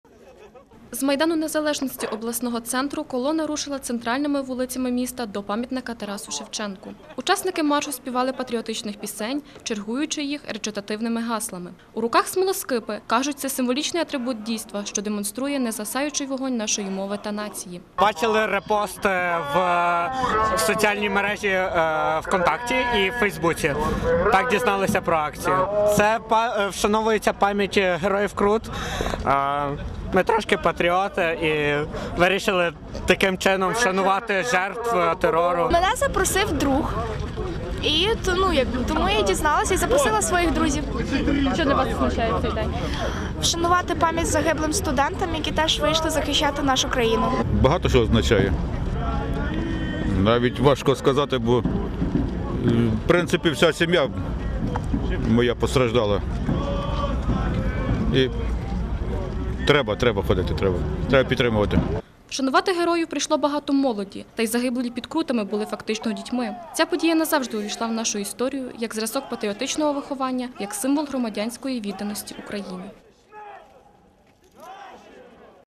З вогнем та піснею хмельничани пройшли смолоскипною ходою
У п’ятницю, 29 січня, патріоти краю  зібралися для спільної ходи зі смолоскипами, аби вшанувати пам'ять Героїв Крут.